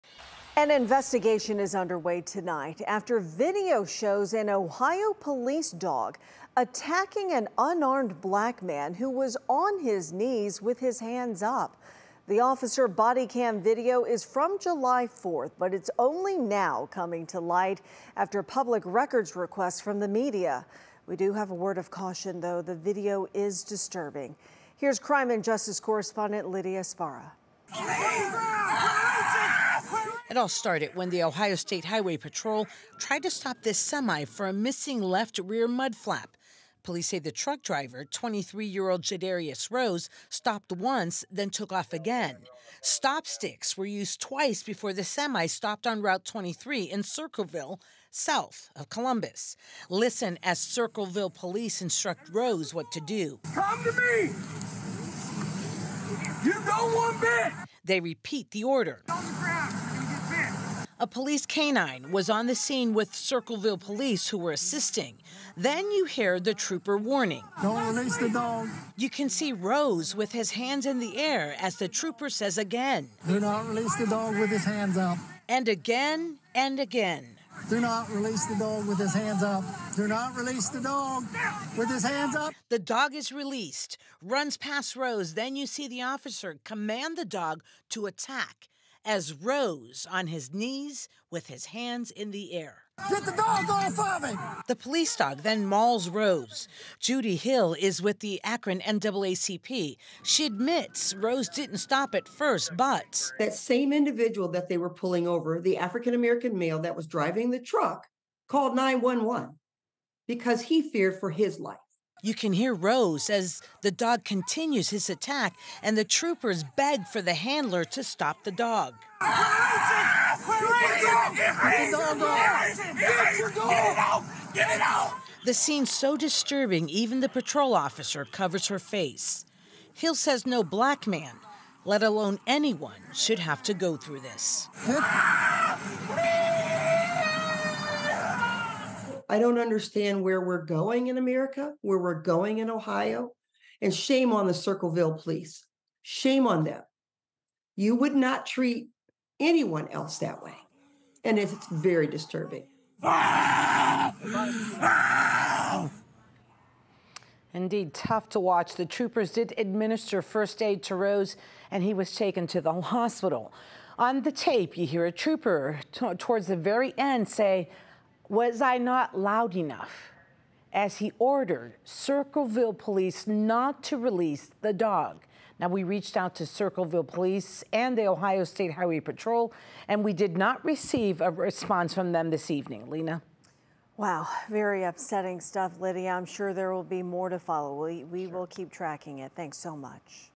Circleville Police Department K-9 attacks semi truck driver surrendering with his hands up
According to an NBC News report, bodycam video has been released showing a Circleville police releasing a K-9 at a semi-truck driver during a traffic stop after another trooper could be heard telling the officer not to release the dog.

The incident originally happened on Tuesday, July 4 on U.S. Route 23 after Ohio State Highway Patrol troopers engaged in a "lengthy" pursuit of 23-year-old semi-truck driver Jadarrius Rose. Officers attempted to initiate the traffic stop after his vehicle was observed to be missing a left rear mud flap.

After failing to yield initial attempts to stop his vehicle, stop sticks were used twice before the vehicle stopped. According to NBC News' report, an incident report from the traffic stop stated, "After several times of being ordered to exit the vehicle, the suspect exited the vehicle from the driver’s side door. The driver was given orders to get down on the ground and the suspect would not comply."